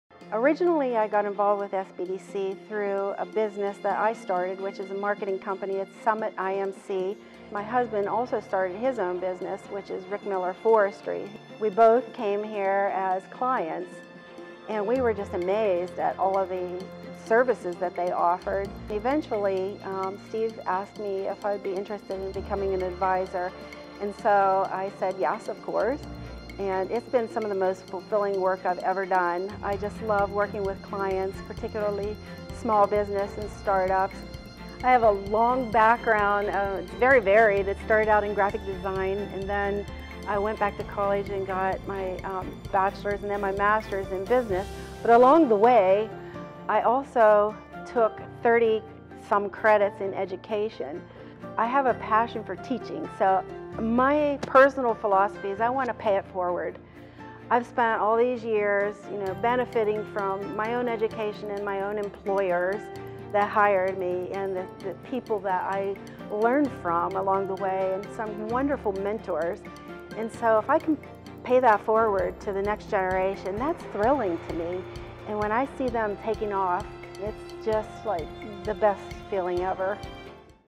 SBDC Advisor, Cyndi Miller
Cyndi specializes in marketing with a focus on:
• Website Design & Development
• Press Releases & Media Relations
• Digital Marketing
• Lead Development & Mailings
• Customer Database Management
• Branding & Promotion
• Marketing/Advertising Strategies
• Market Development & Research
• Business Start-Up Considerations (i.e., Trademarking, Pricing, Packaging Labeling, Shipping, etc.)
• Trade Show Development
• Event Planning